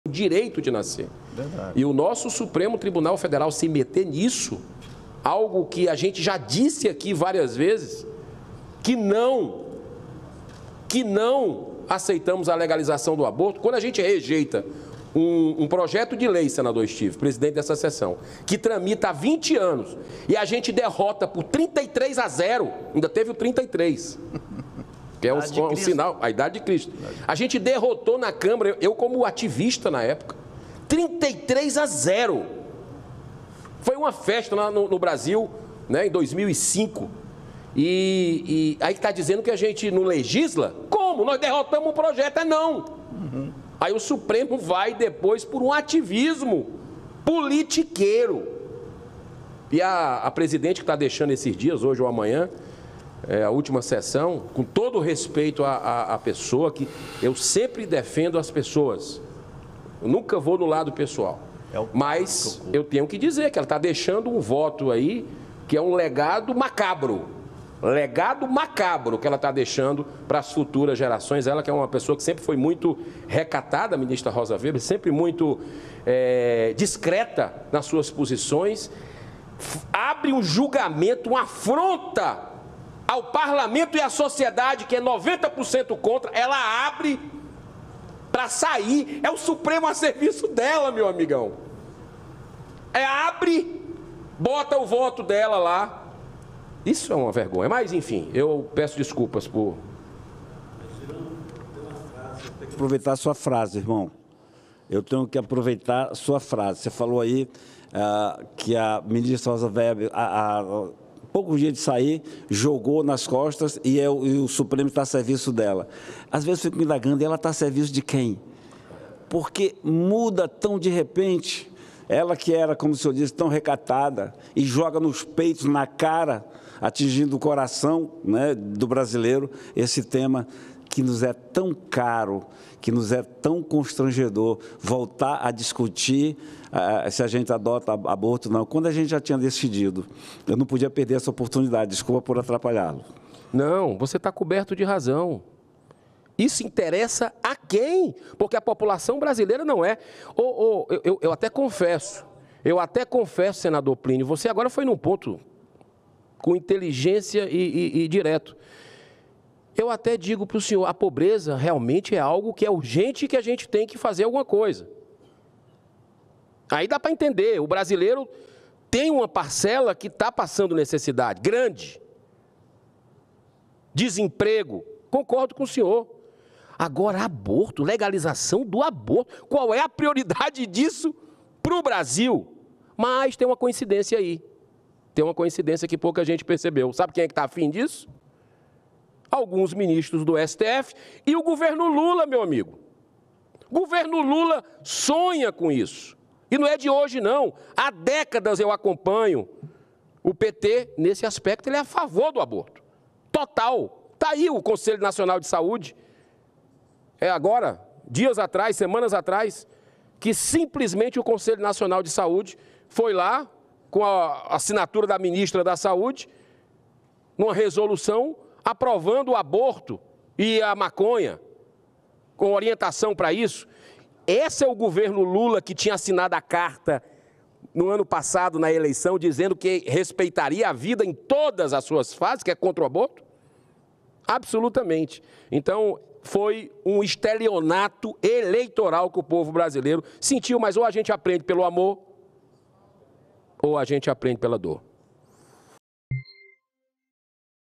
Girão sobre Rosa Weber: 'Deixando um legado macabro'
Durante Sessão Deliberativa do Plenário nesta quarta-feira (27/9), o senador Eduardo Girão (Novo) citou a aposentadoria da ministra do STF, Rosa Weber, e criticou o voto dela sobre a liberação do aborto no Brasil. Para o parlamentar, a ministra está "deixando um legado macabro".